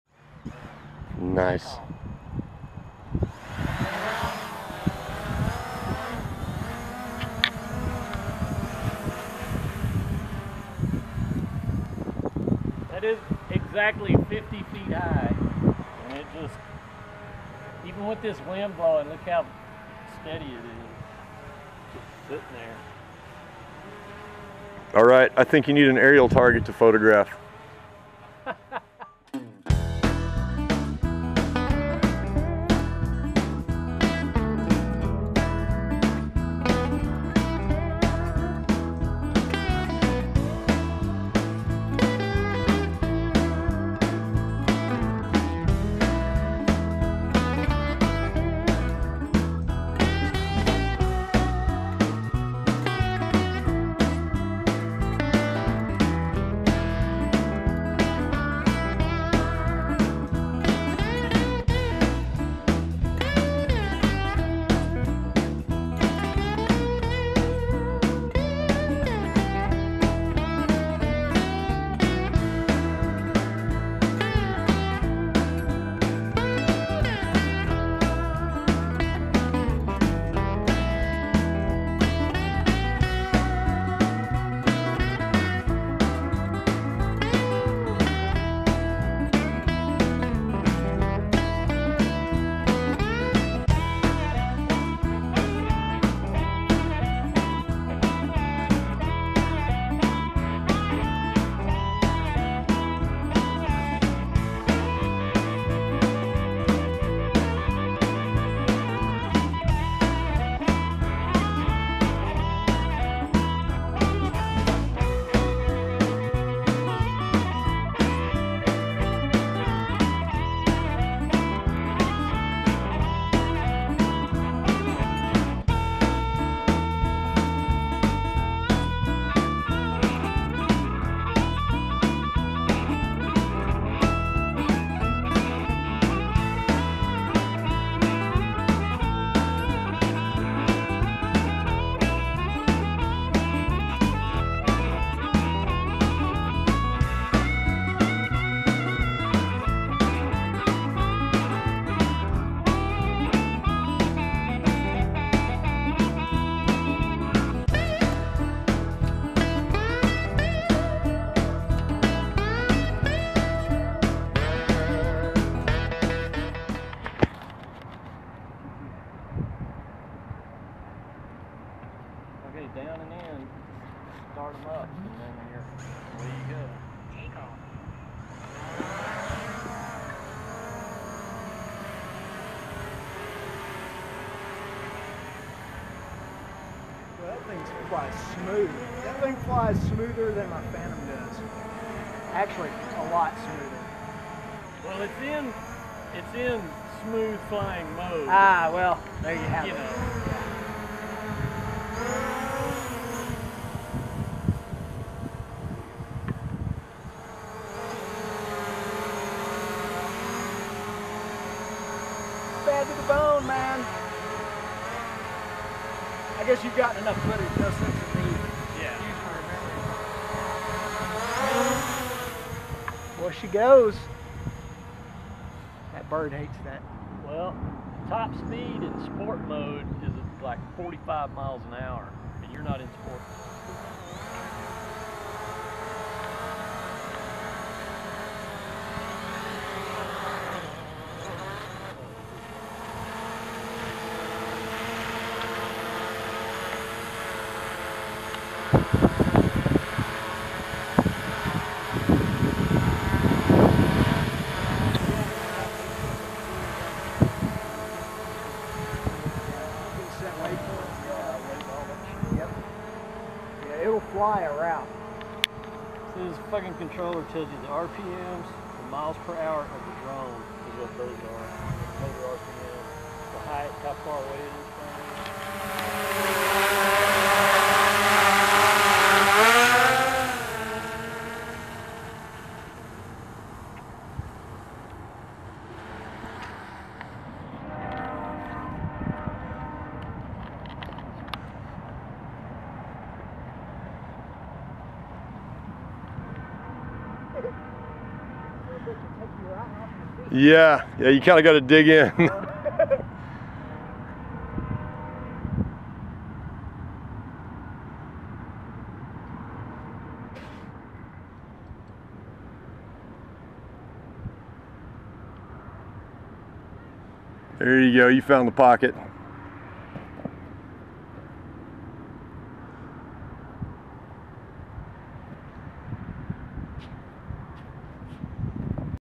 Drones and Kites
Clif invited me and Tim along for the maiden flight of his new DJI drone, and I brought the 2.6 meter Peter Lynn kite along for something to photograph with it.  A couple of shots are blurred, but that's because I constantly forget to check the settings on the DSLR I've used for years.  Anywho, the drone footage is nice, and Tim got to fly a power kite for the first time.